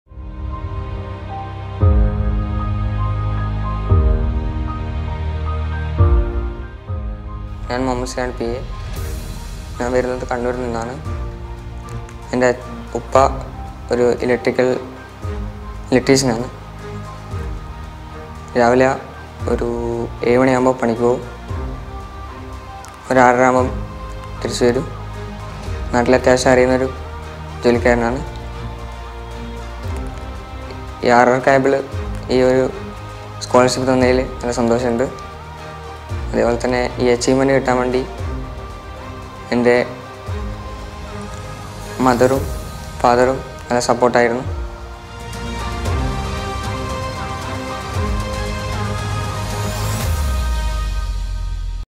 Kabel Star Muhammed Sinas - Calicut
Congratulations to our Kabel Stars from Calicut! Hear them speak about the Kabel star scholarship and their bond with RR Kabel.